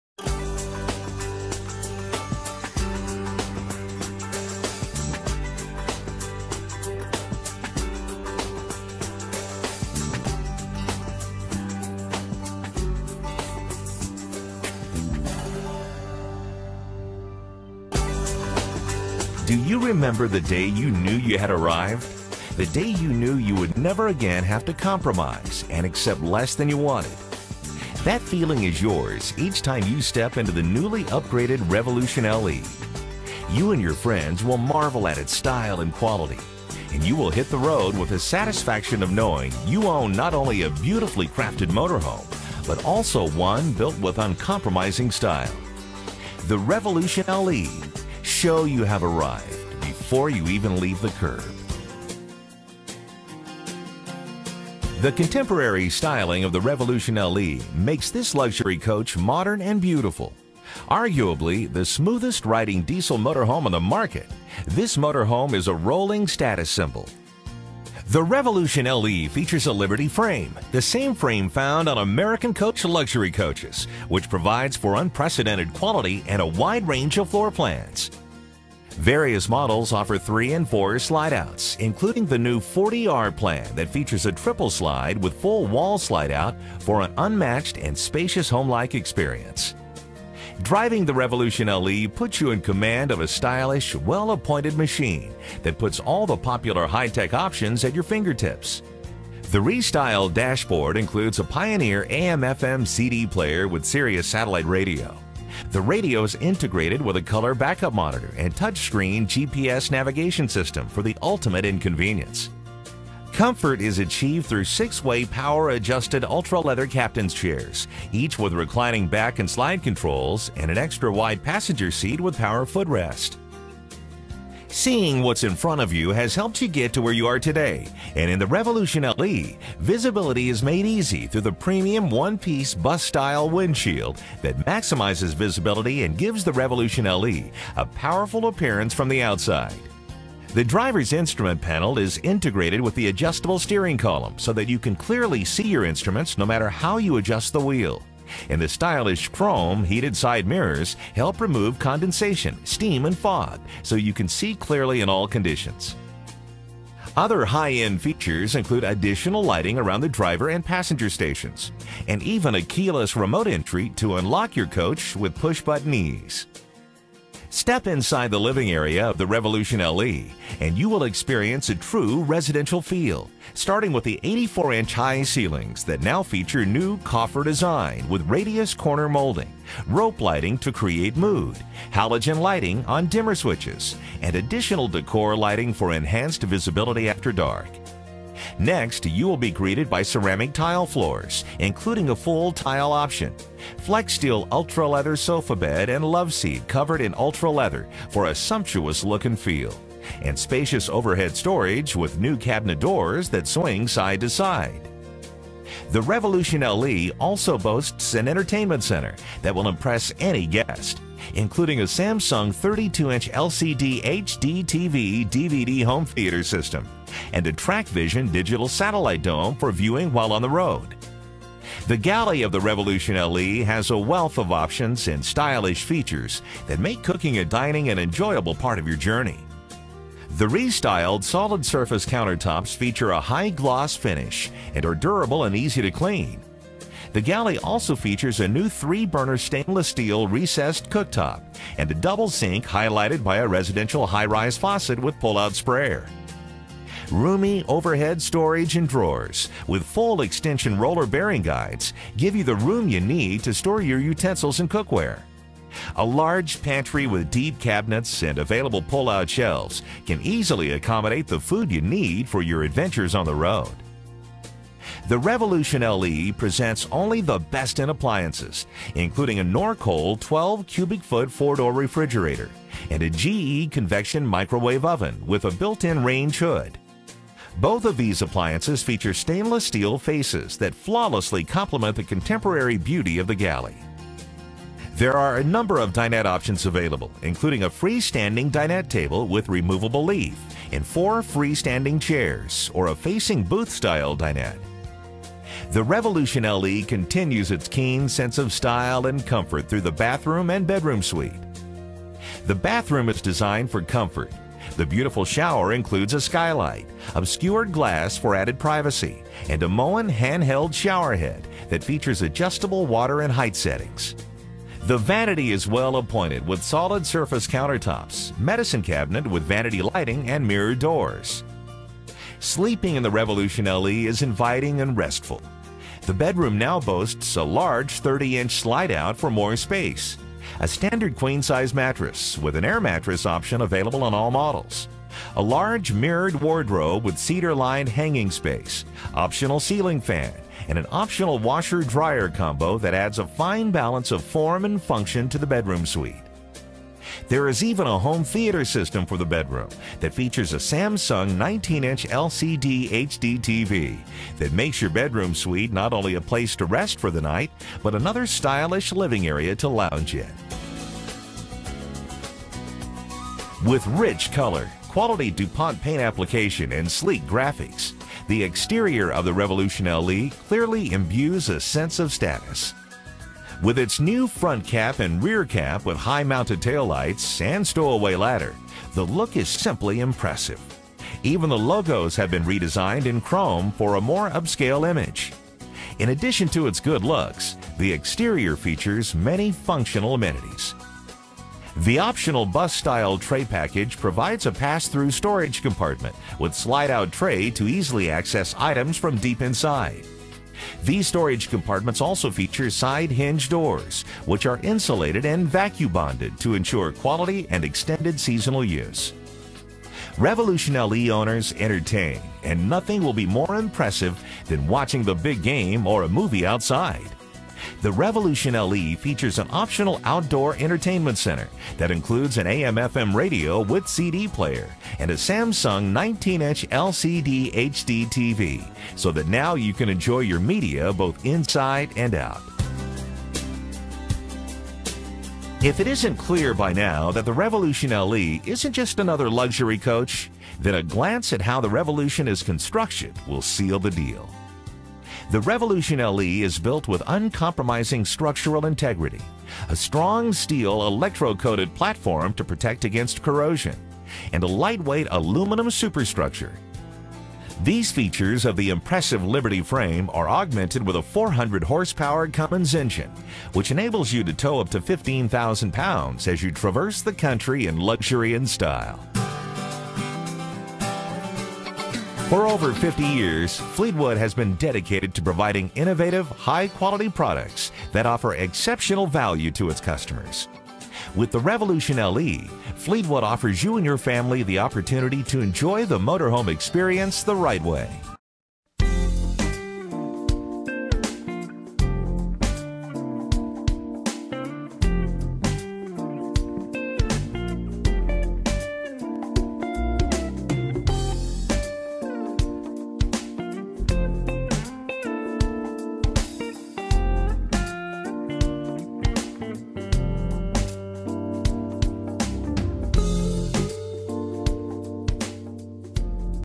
2008 Fleetwood Revolution LE motorhome
2008 Revolution diesel pusher motorhome from Fleetwood RV. This motorhome is on sale at North Trail RV Center in Fort Myers, Florida. Excellent video showcases all features and options on this great motor coach.